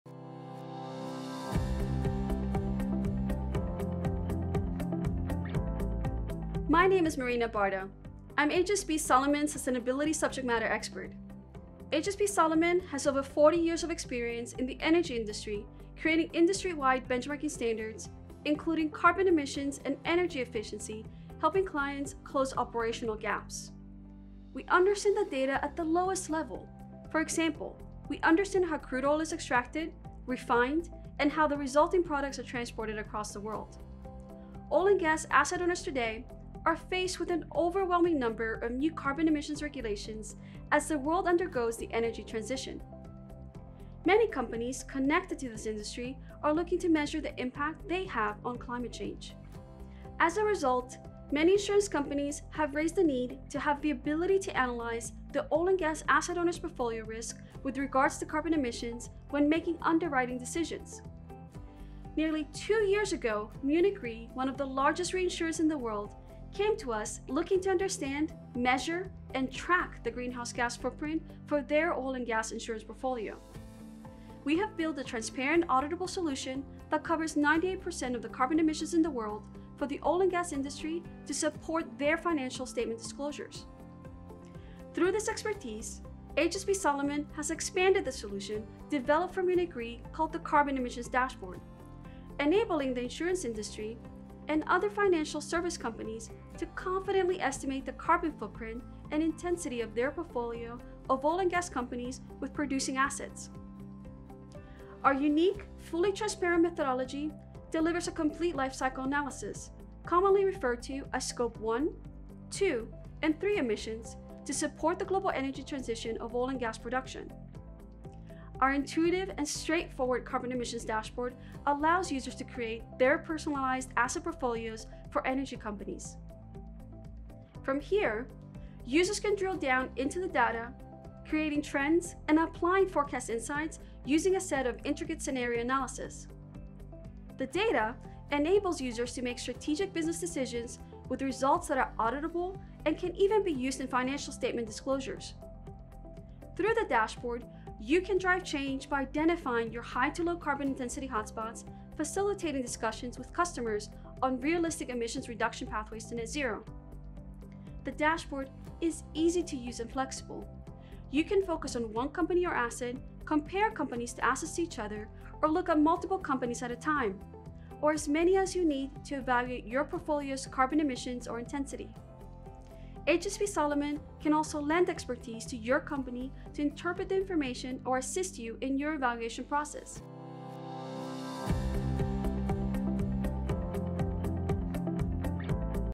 Why Solomon? Why the Carbon Emissions Dashboard?
HSB Solomon has over 40 years of experience in the Energy Industry creating industry-wide benchmarking standards including carbon emissions and energy efficiency and helping clients close operational gaps. We understand the data at the lowest level.

Today, many companies are looking to measure the impact they have on climate change.  As a result, many insurance companies have raised the need the have the ability to analyze the Oil & Gas asset owners portfolio risk with regards to carbon emissions when making underwriting decisions. We built the Oil and Gas Carbon Emissions Dashboard as a transparent, auditable solution that covers 98% of the carbon emissions in the world for the Oil & Gas industry, to enable the insurance industry and other financial service companies to confidently estimate the carbon footprint and intensity of their portfolio of Oil & Gas companies with producing assets.